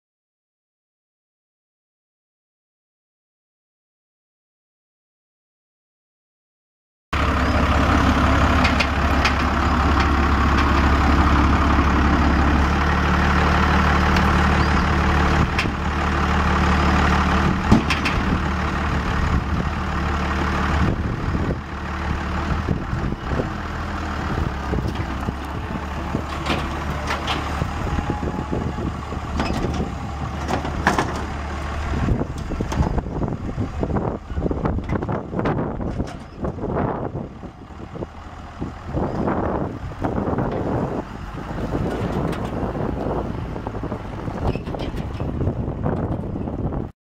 1996 Dodge Ram 3500 LT pickup truck for sale | sold at auction October 30, 2013
1996 Dodge Ram 3500 LT pickup truck
170,204 miles on odometer
5.9L L6 OHV 20V turbo diesel engine
Five speed manual transmission
Two wheel drive
AC and heat
Tilt
Cruise
AM/FM/cassette
Power windows and locks
Cloth interior
8&#039; dump bed
81"W
Manual end gate
PTO driven
Hiniker 8&#039; snow plow
12"W extensions
Two kick back springs
Plow lights
Receiver hitch
Trailer wiring
215/85R16 tires
1B6MC36C5TJ123386

It was located in Webb City, MO, and we sold it to the highest bidder for $7,810.